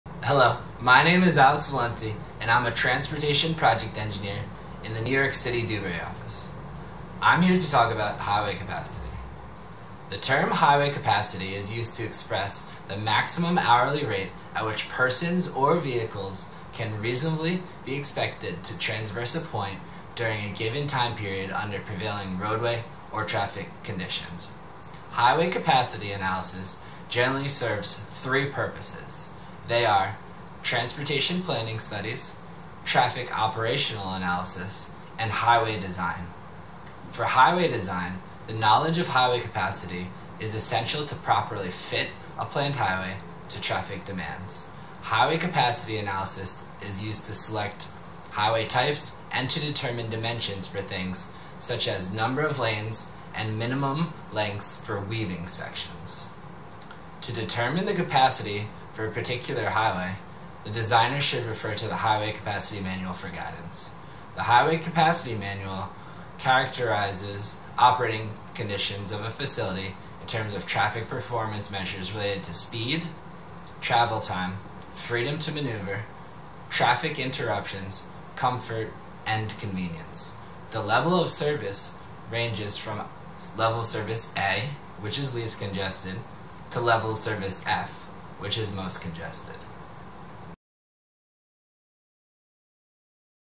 What is Highway Capacity?
Transportation Engineer Alex Valenti explains highway capacity in about 90 seconds.

Hello. My name is Alex Valenti and I'm a transportation project engineer in the New York City Dewberry office. I'm here to talk about highway capacity. The term highway capacity is used to express the maximum hourly rate at which persons or vehicles can reasonably be expected to transverse a point during a given time period under prevailing roadway or traffic conditions. Highway capacity analysis generally serves three purposes; they are transportation planning studies, traffic operational analysis, and highway design.

For highway design, the knowledge of highway capacity is essential to properly fit a planned highway to traffic demands. Highway capacity analysis is used to select highway types and to determine dimensions for things such as number of lanes and minimum lengths for weaving sections. To determine the capacity for a particular highway, the designer should refer to the highway capacity manual for guidance. The highway capacity manual characterizes operating conditions of a facility in terms of traffic performance measures related to speed, travel time, freedom to maneuver, traffic interruptions, comfort, and convenience. The level of service ranges from level service A, which is least congested, to level service F, which is most congested.